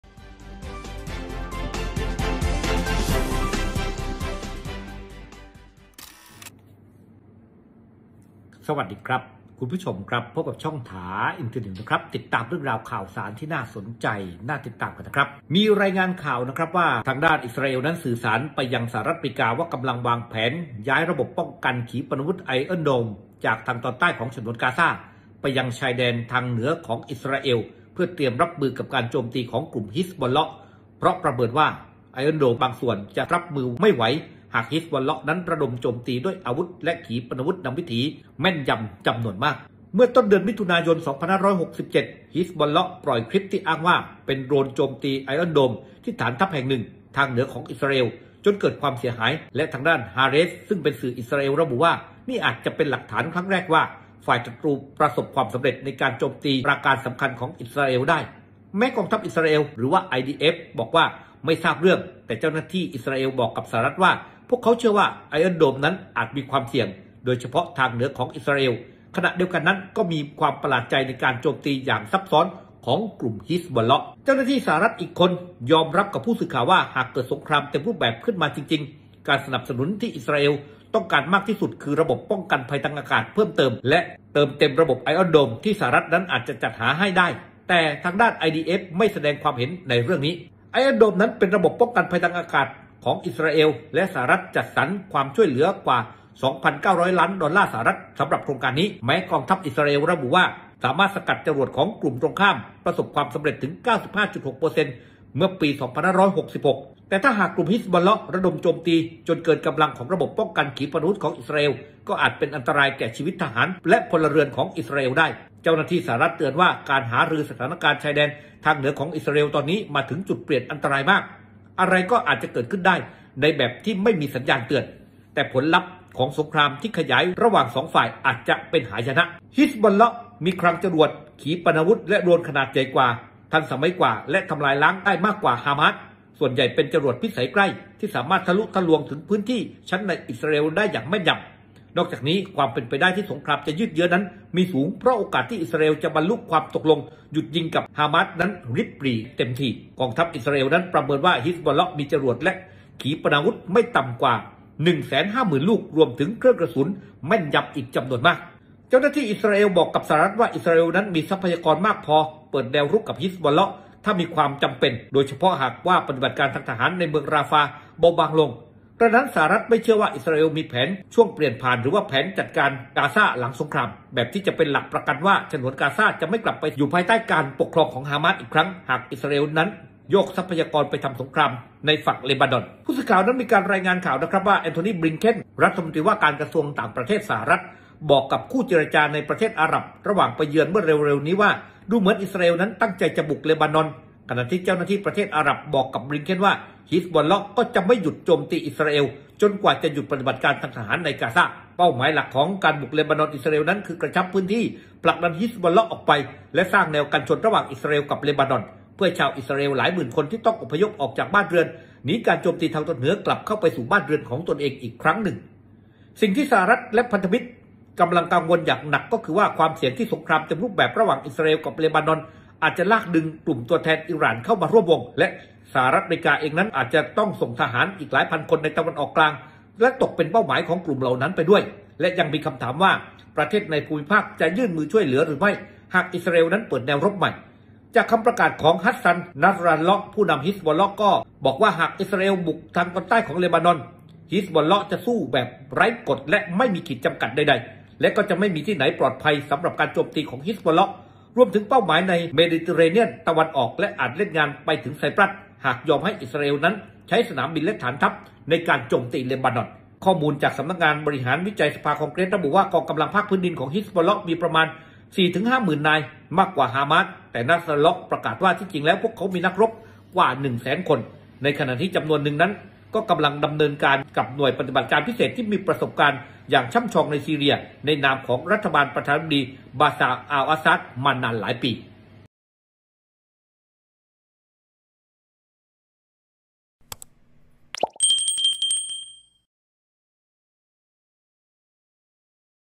"สหรัฐฯ" หวั่น ไอเอิร์น โดม อิสราเอล รับไม่ไหว หากโดนฮีชบอลเลาะห์ ถล่ม | THA Internews
สหรัฐฯ กำลังวิตกว่า กรณีที่เกิดสงครามระหว่างอิสราเอลกับฮิซบอลเลาะห์เต็มรูปแบบ ระบบไอเอิร์น โดม ที่อยู่ทางเหนือของอิสราเอล จะรับมือการโจมตีไม่ไหว